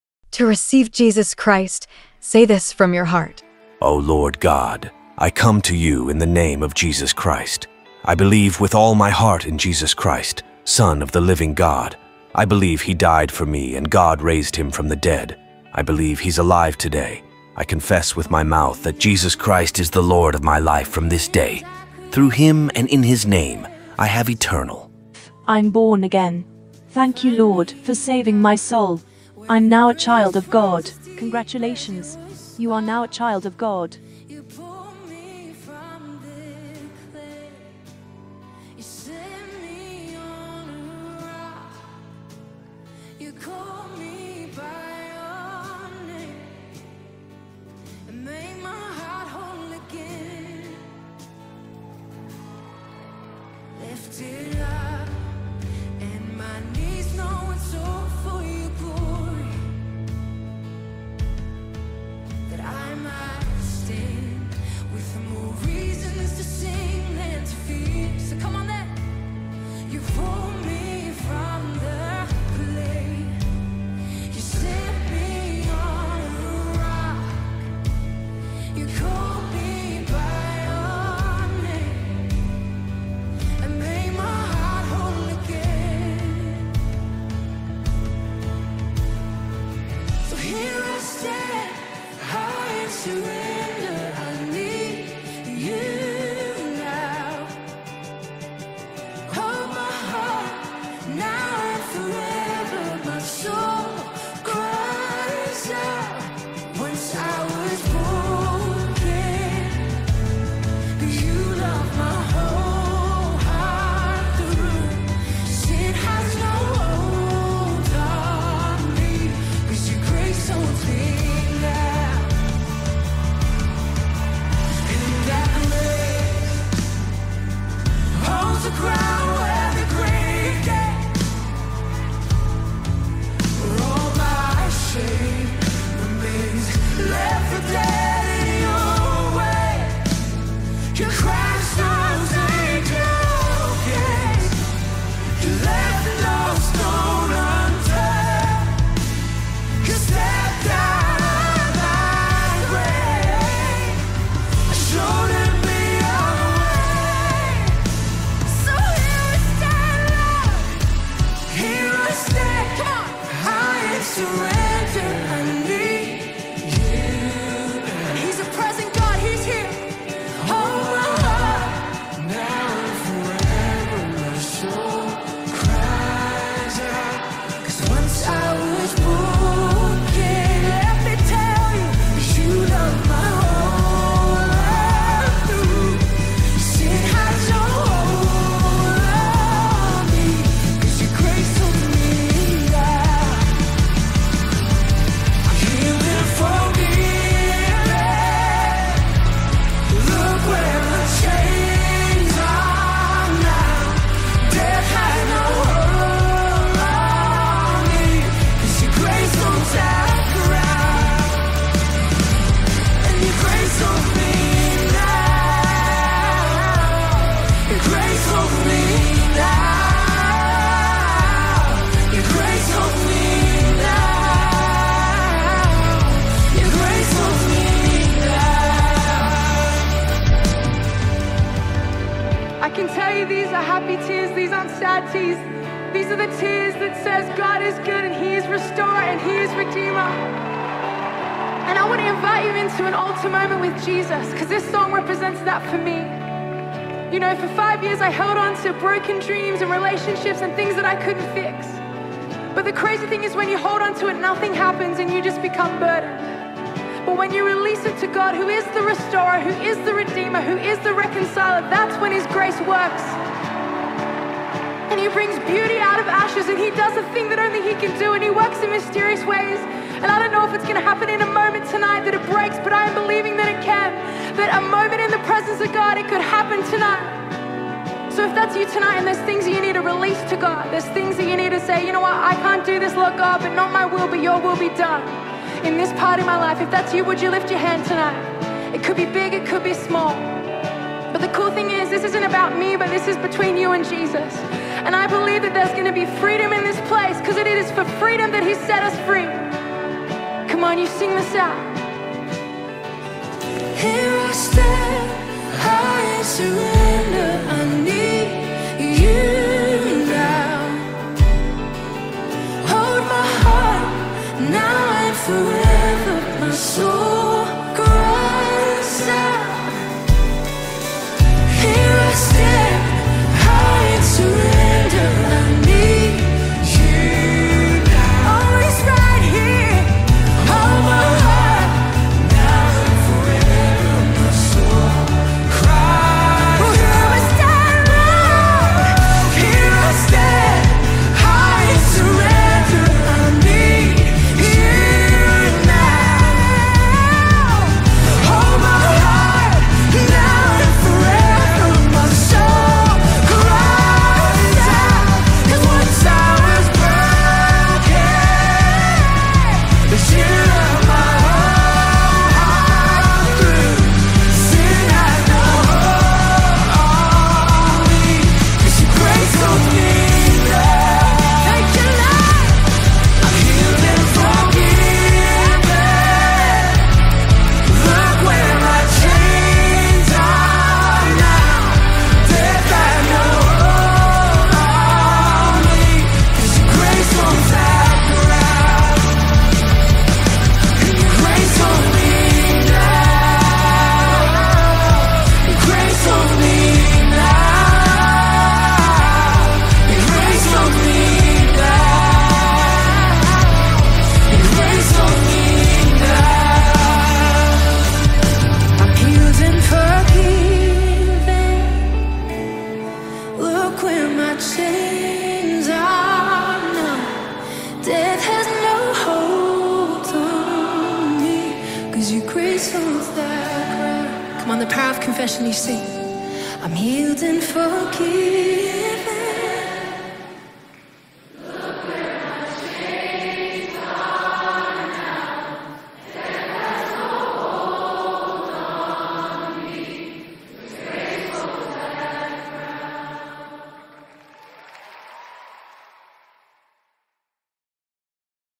Confess this to receive Jesus Christ Salvation Prayer×Whole heart
You are free from your PAST, you are now a new creature, now FIND A CHURCH, SPEND TIME in the world of GOD,PRAY TO HAVE A BETTER REVELATION AND RELATIONSHIP with GOD.